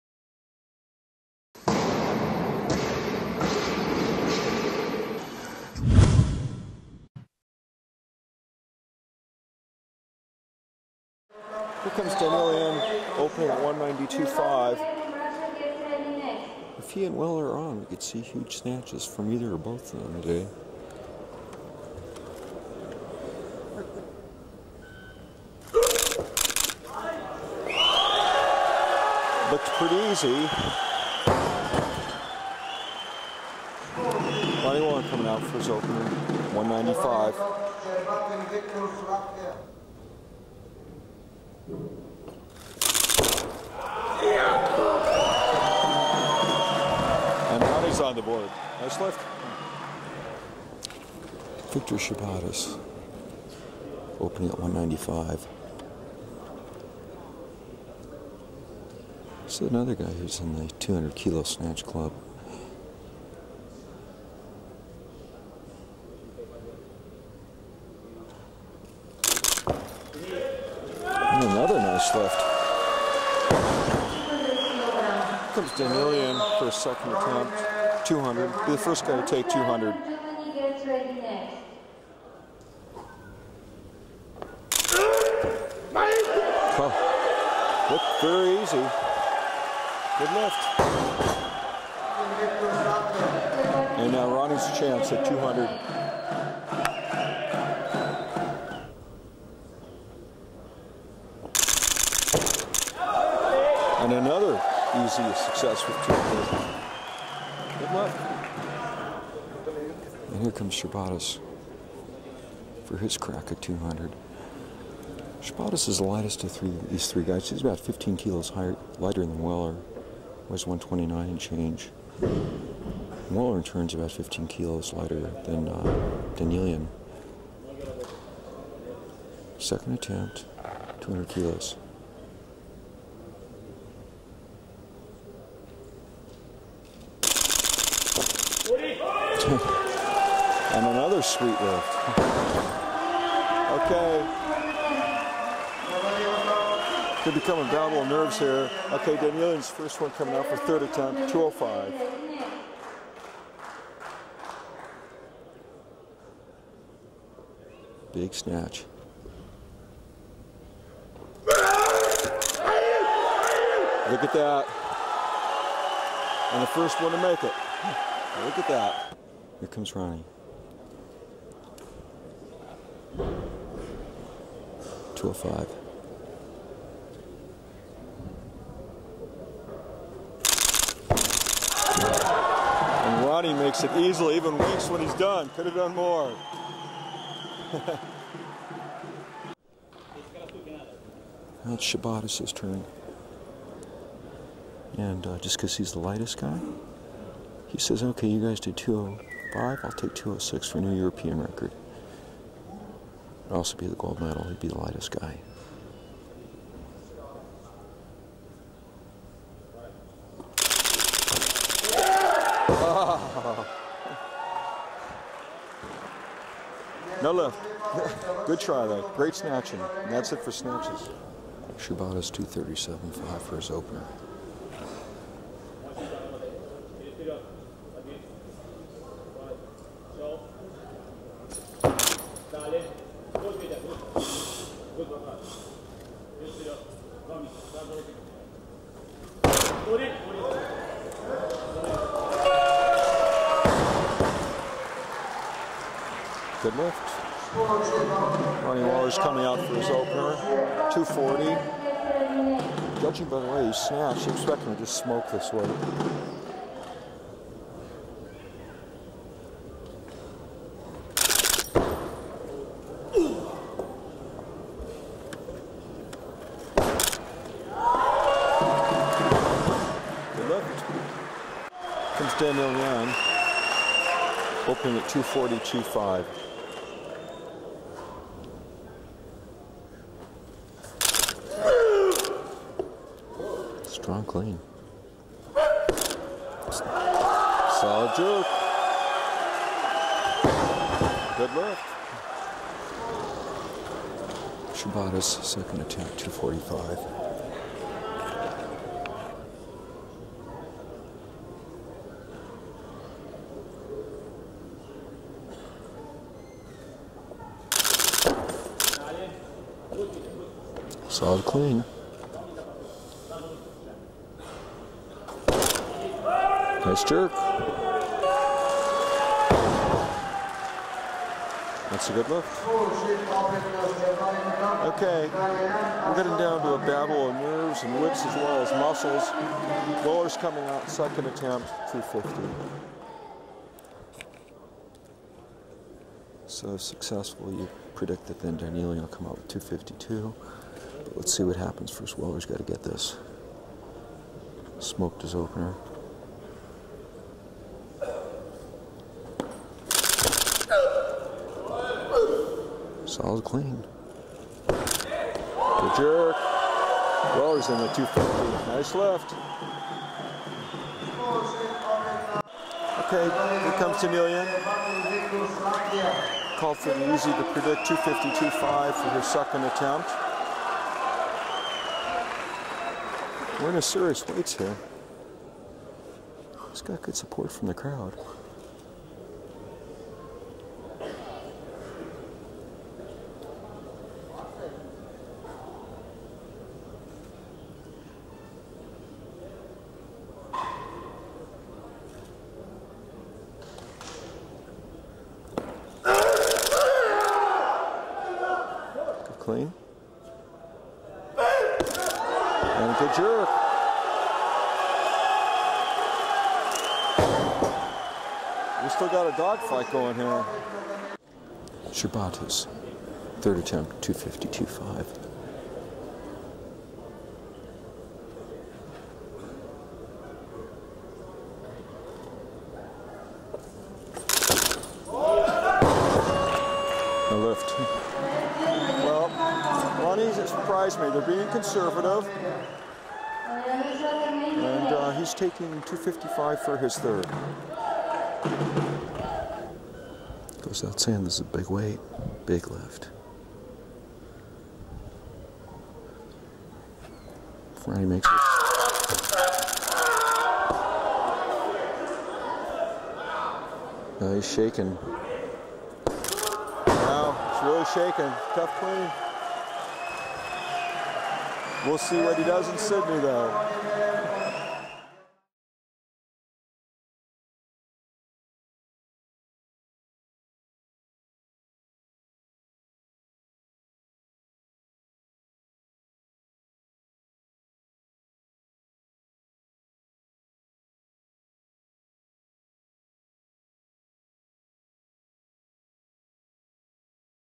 IronMind Big Lift Series: Danielyan - Weller - Scerbatihs
It was clear that the supers had entered a new era—and big weights were lifted in Sofia, Bulgaria at the 2000 European Weightlifting Championships. Ashot Danielyan (Armenia) opened with a routine 192.5-kg snatch. Ronny Weller (Germany) opened with a nice 195, as did Victors Scerbatihs (Latvia). On the board, all three men were then poised for a run at +200-kg snatches, which separates the world’s best from all of the rest. About 15 kg separated each on bodyweight, with Danielyan the heaviest and Scerbatihs the lightest. Each made a sweet success with 200 kg on their second attempt. Danielyan had a solid success with the world-class weight of 205 kg on his third, and Weller blistered the 205 and could have done more. Scerbatihs, obviously not intimidated by giving up 15 to 30 kg in bodyweight, decided that 205 looked pretty easy, so he took 206 and looked as if he had it, but didn’t quite make it. Scerbatihs wasn’t expected to keep up with Danielyan and Weller in the clean and jerks but he opened with a solid 237.5 kg. Weller came back with 240 kg, considered a threshold weight for a top super, and Danielyan responded with a very solid 242.5 kg to cover the bodyweight gap. We were set up now for a great race to the finish, and Scerbatihs, calling for 245 on his second, made another nice lift. Weller was the first to take things to the next level, calling for 250 and nailing it with what looked like room to spare. Danielyan took a predictable 252.5 kg which he made without undue effort. Scerbatihs took the same 252.5 on his third for what would have been an excellent lift for himself, but lost the weight when racked—it just bounced out. In a surprise move, Weller called for 255 on his third attempt. He racked the bar without too much trouble, but had to take two steps forward after he recovered from the clean. Weller might have been passing out at that point as he was shaking, but he tried to make the jerk anyway, only to miss it—which meant that Danielyan had just won with a total of 457.5 kg, so he passed on his third attempt. Relive the excitement with this exclusive IronMind footage.  For more on this story, see MILO, September 2000, Vol. 8, No. 2 in the IronMind e-store